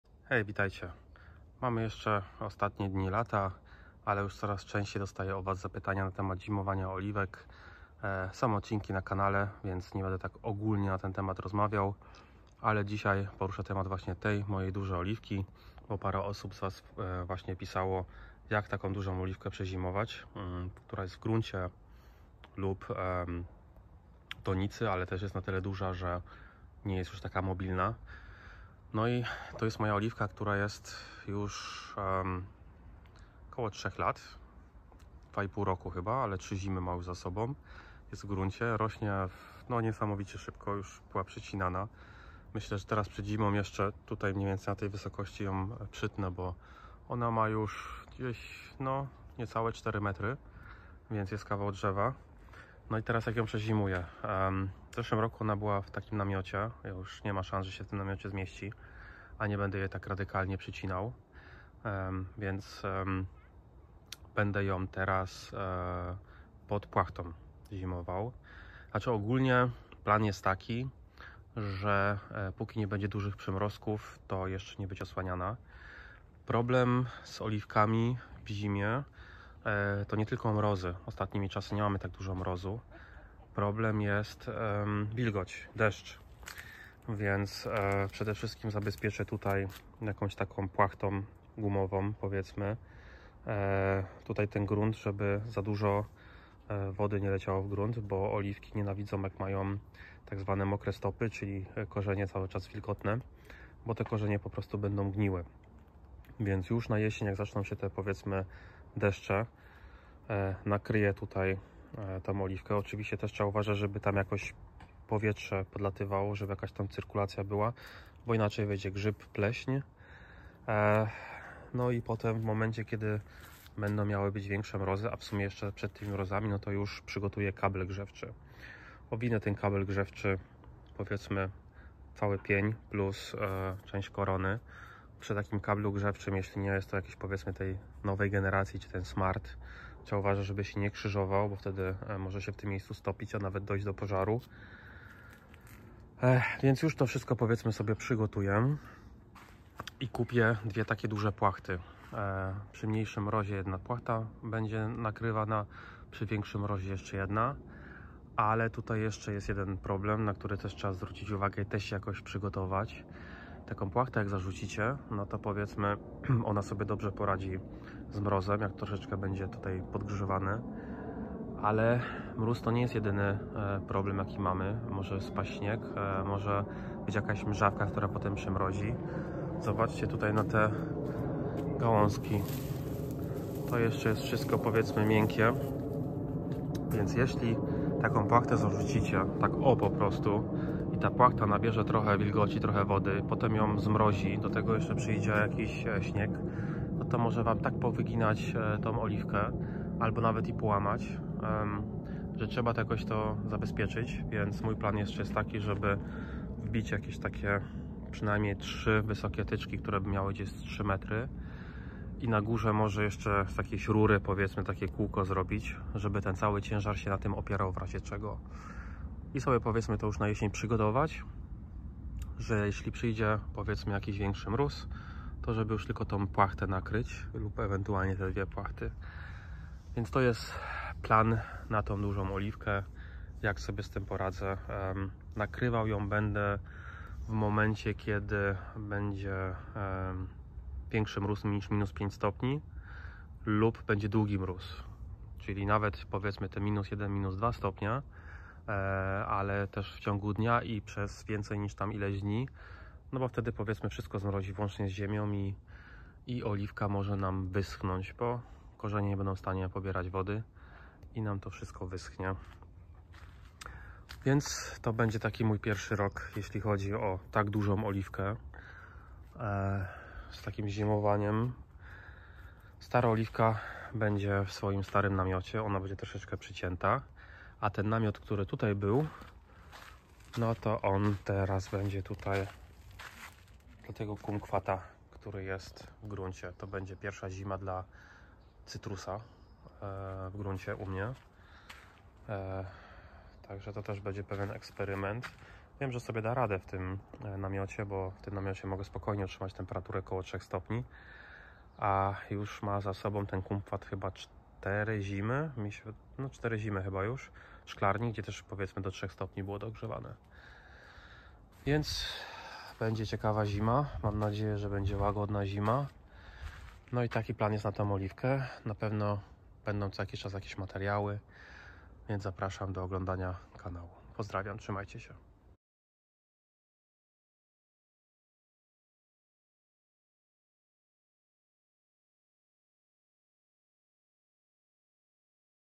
Odc. 332 - Jak będę w tym roku zimował moją wielka oliwkę.
Jeśli uważasz, że ten material był interesujący i chcesz wesprzeć ten kanał ...postaw mi kawę :)